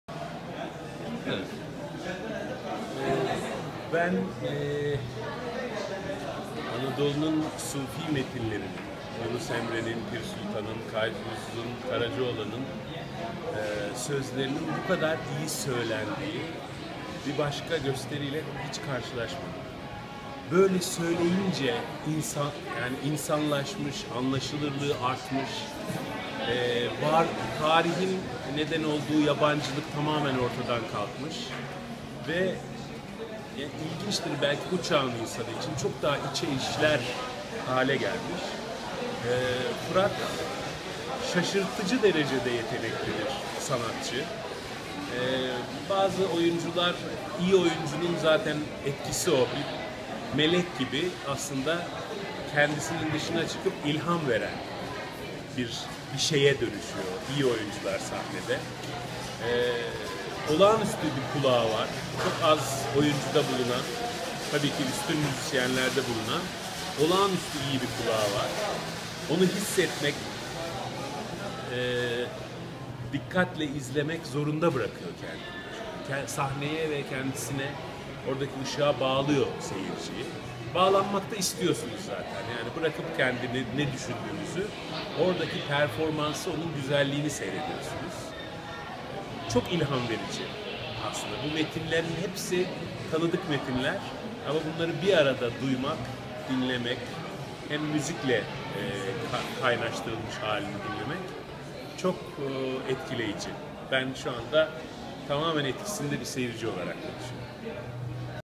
Ezel Akay "Gelin Tanış Olalım"ı anlatıyor.
Fırat Tanış ile Gelin Tanış Olalım Türkülü Oyun 12,19,26 Ağustos 2016 tarihlerinde Trump Gösteri Merkezi'nde izlenebilir.